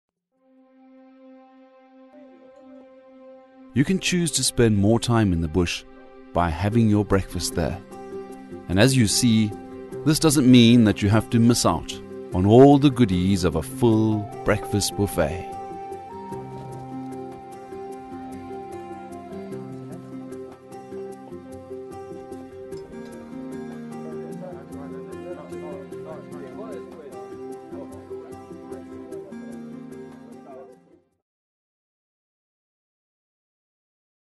Shamwari Game Reserve Bush Breakfasts, Eastern Cape - South Africa
Eating breakfast in the bush does not mean you have to miss out on all the goodies of a full buffet breakfast!
This video was produced by SmartGuide for their online video guide.